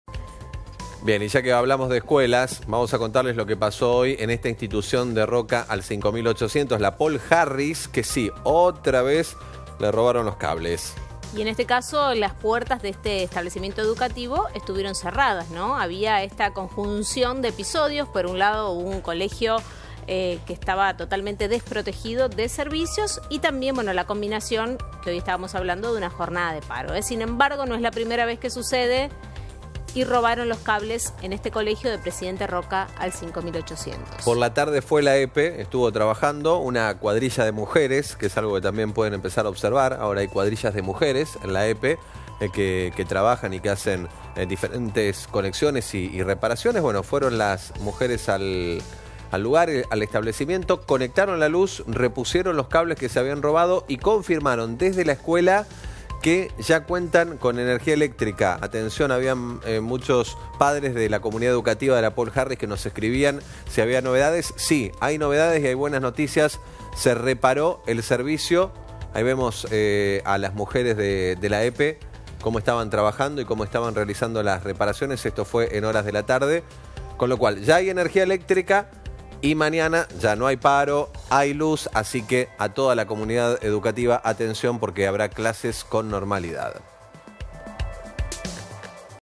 El drama de los vecinos tras el incendio del ascensor | Telefe Rosario
Está mañana debieron suspender las clases por la falta de luz. Por la tarde trabajó una cuadrilla femenina de la EPE y se pudo reconectar el servicio.